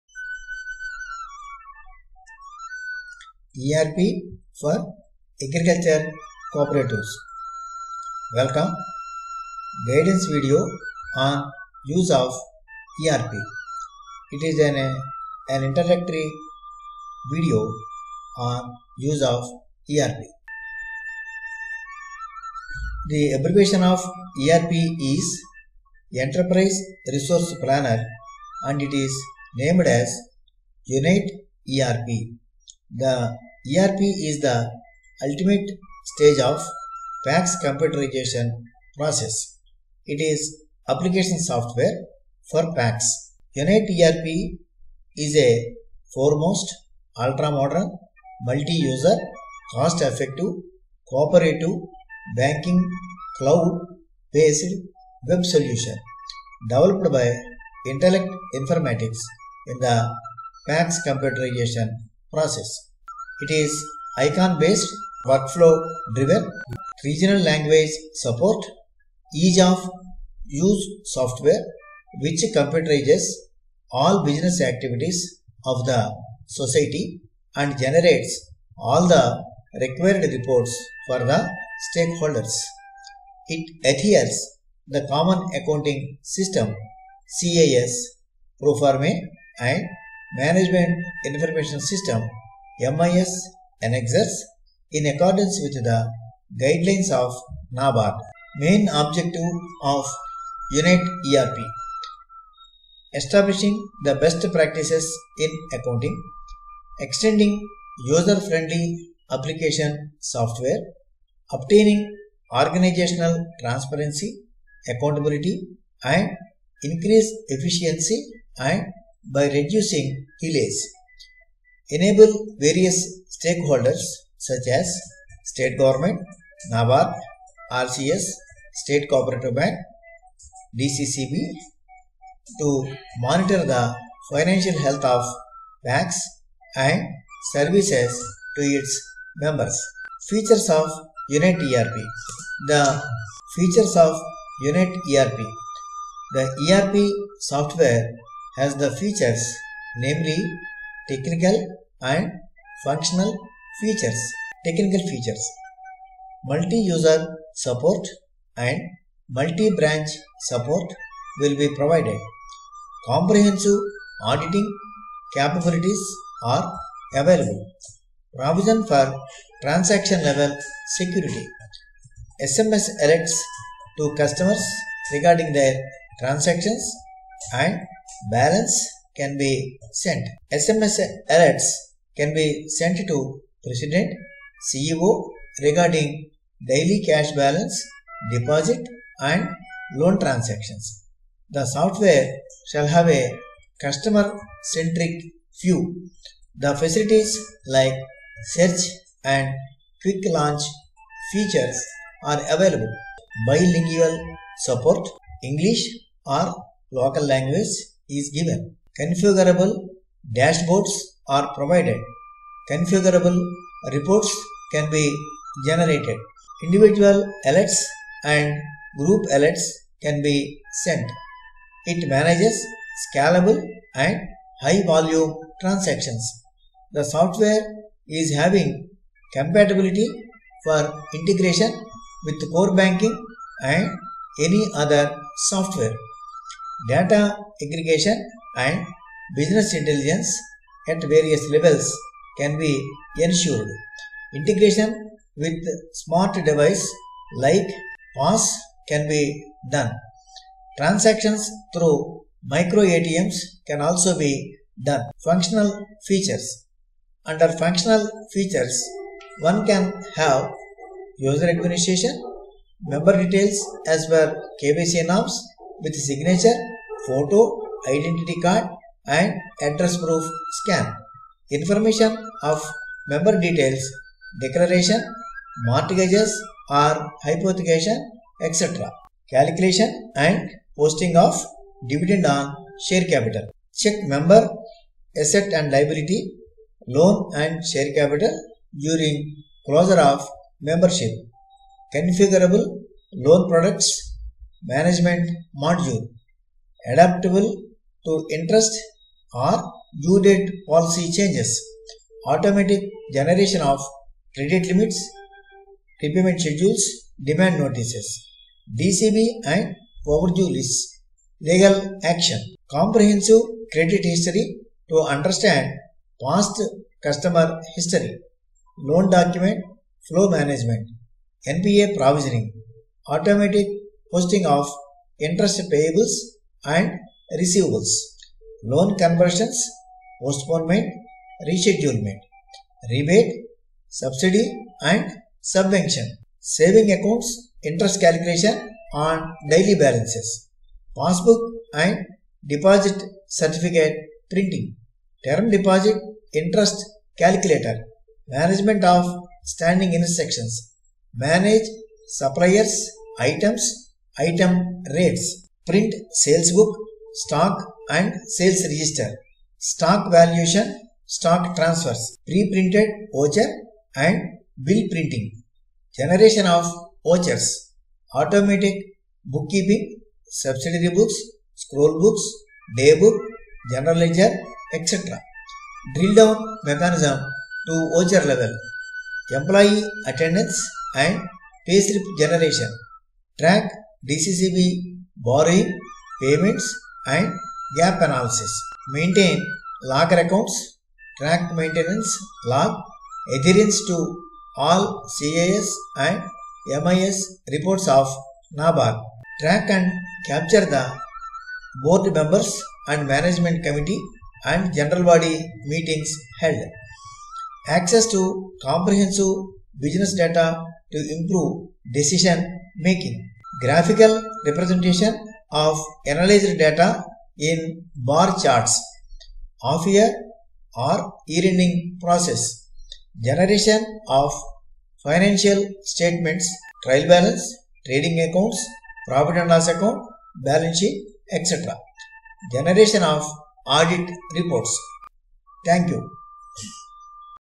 ERP - Introduction
The video introduces the ERP software and its significance.